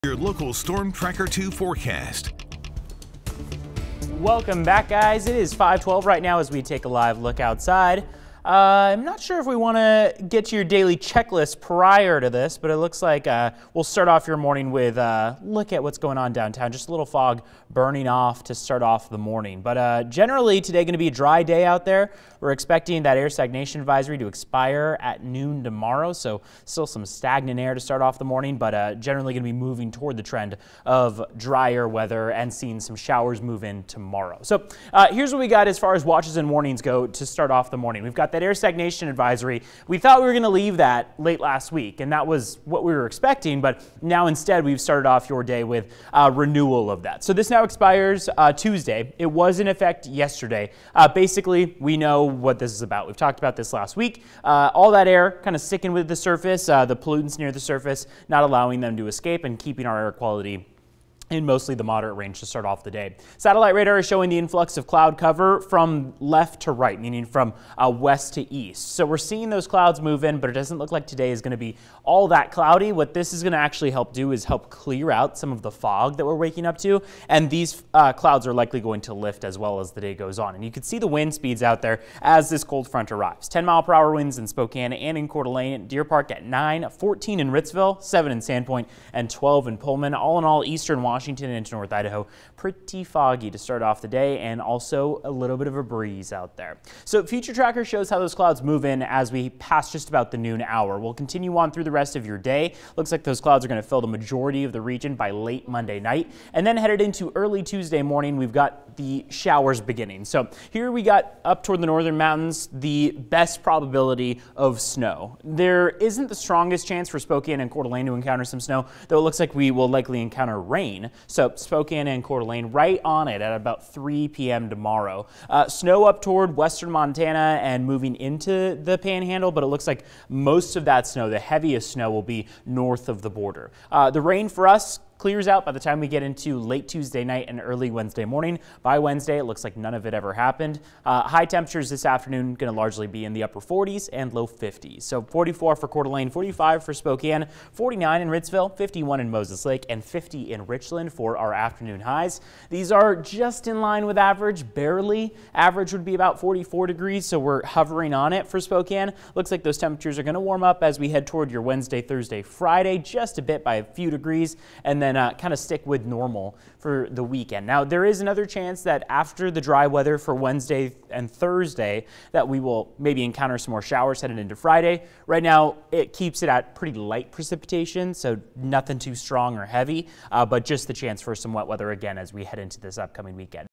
Weather Update 5 a.m., Nov. 11, 2019
KREM's Evan Noorani has the forecast for Spokane, Eastern Washington, and North Idaho for Monday, Nov. 11, 2019.